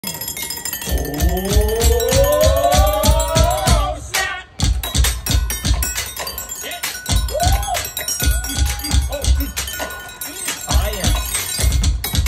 The Beat Builders at Universal Studios Florida really know to jam out!
The Beat Builders is a pop up show at Universal Studios Florida where percussionists use all sorts of tools to create sick beats.